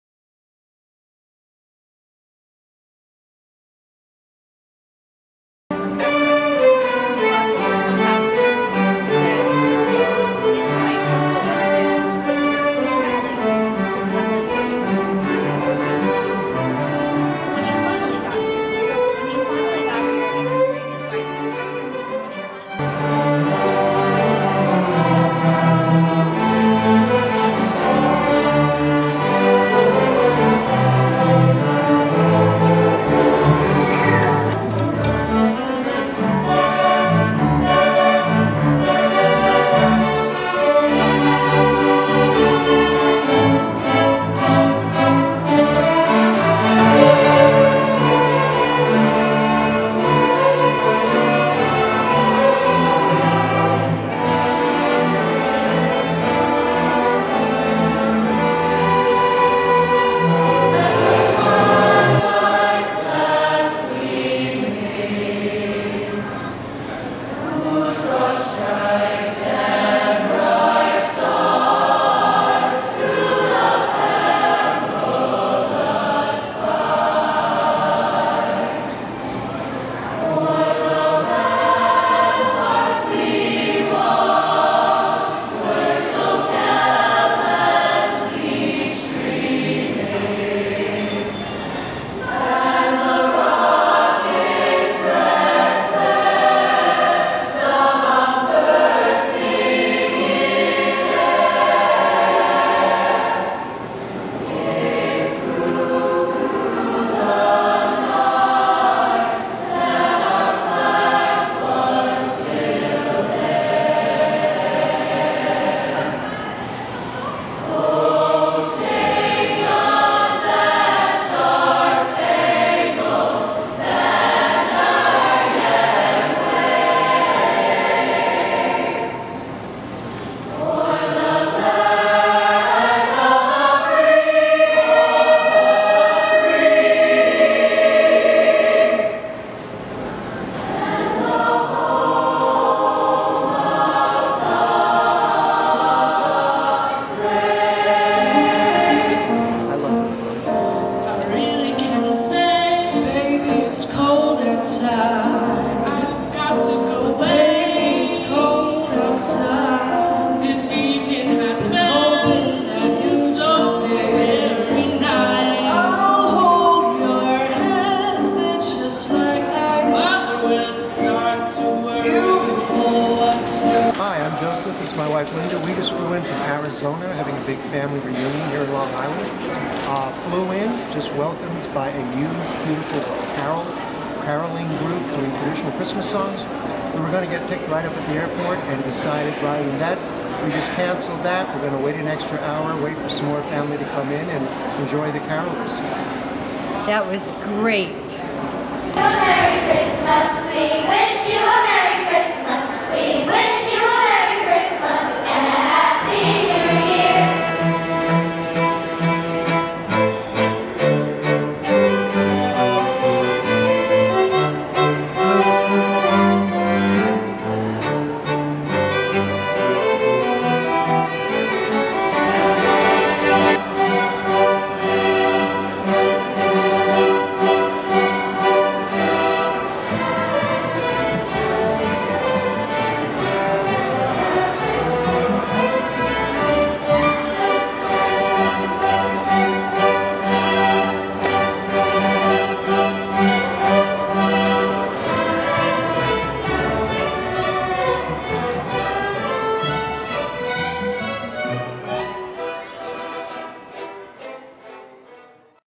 MacArthur Airport's Fourth Annual Concert Series featuring local school performances
19-23 in the Main Terminal Atrium. Six different local schools performed over the course of the five-day event that featured more than 20 hours of holiday music for the traveling public.